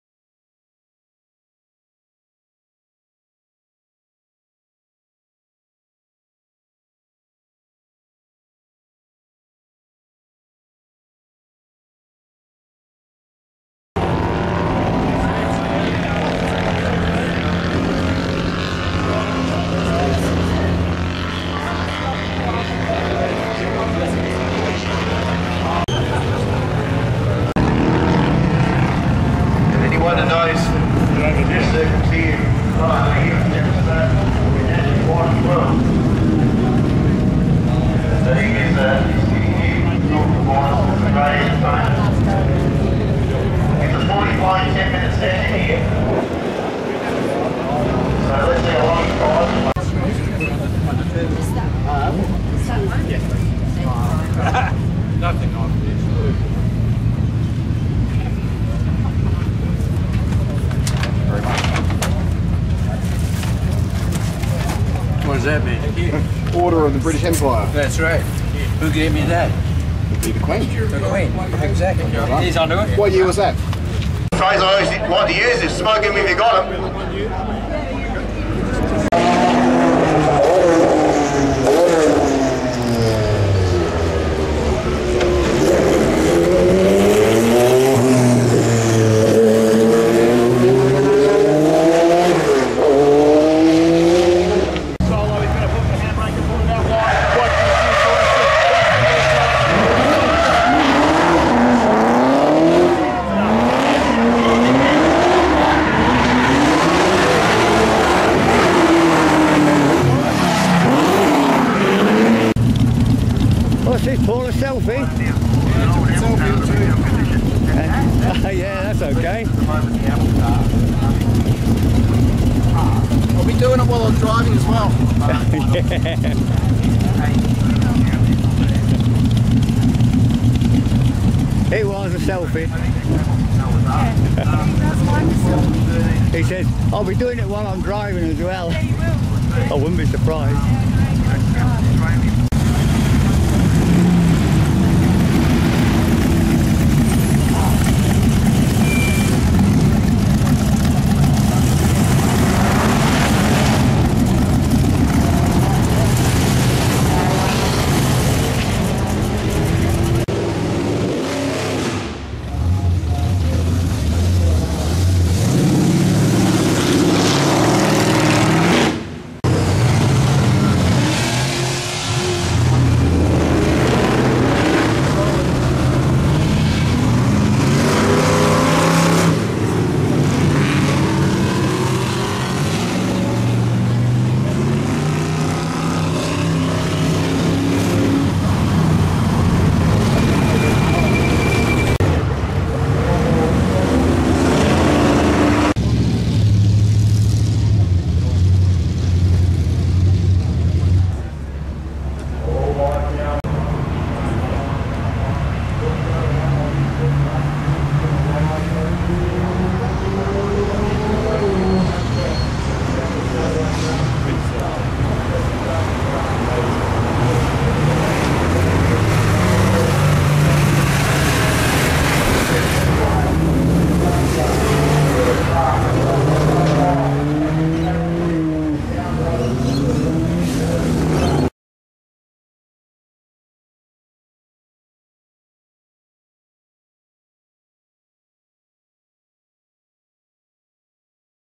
Adelaide Motorsport festival
Paul, Dianne & Pete visit the Adelaide Motorsport festival